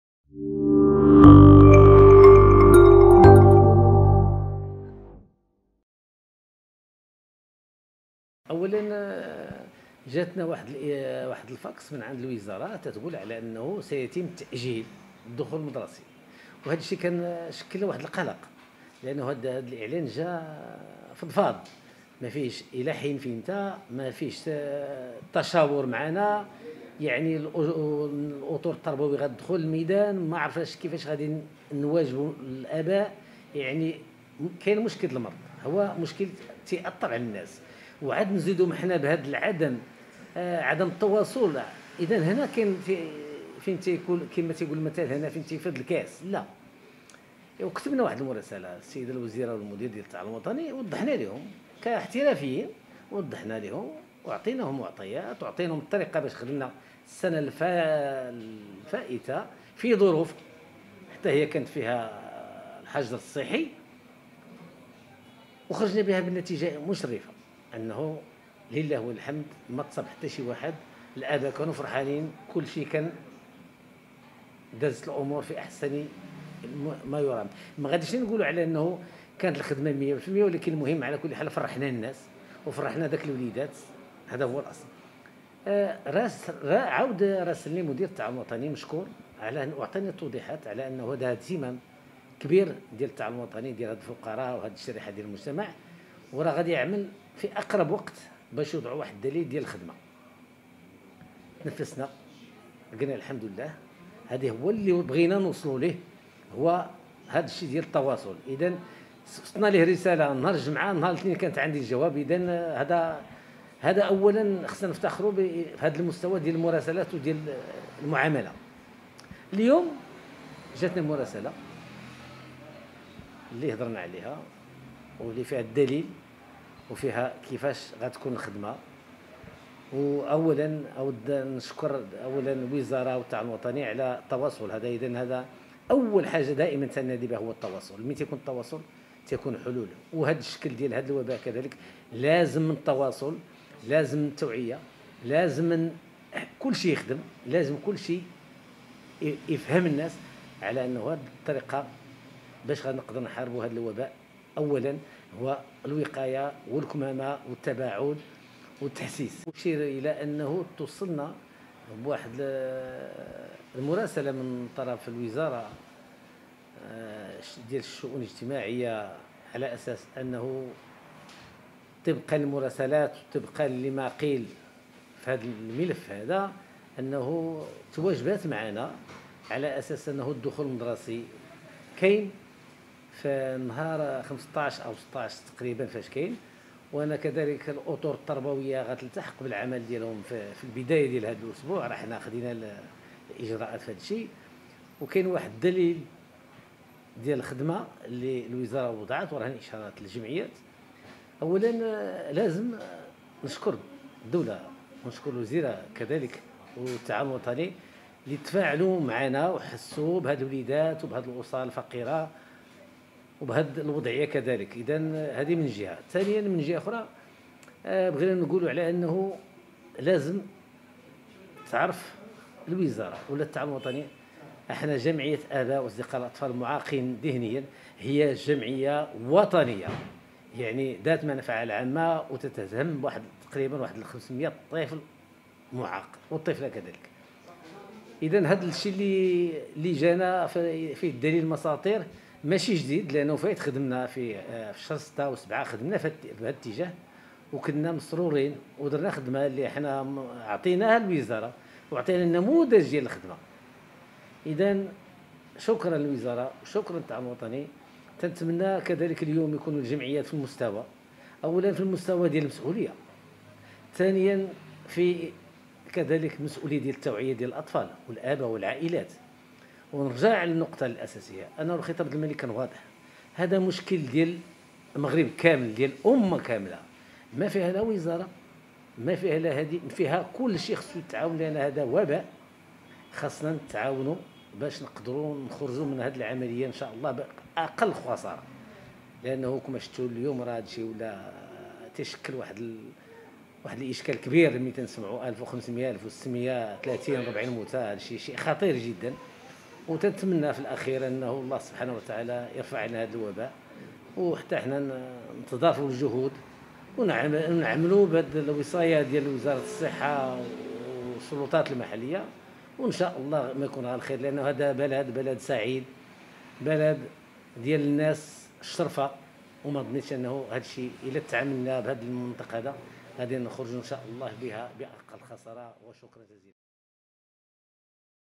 علي رضوان : نعتمد البروتوكول الذي أصدرته الوزارة الوصية من أجل ضمان دخول تربوي واجتماعي آمن
علي رضوان رئيس جمعية آباء وأصدقاء الأطفال المعاقين ذهنيا بالدار البيضاء، يؤكد حرص الجمعية على اعتماد  البروتوكول الذي أصدرته الوزارة الوصيةمن أجل  ضمان دخول تربوي واجتماعي آمن.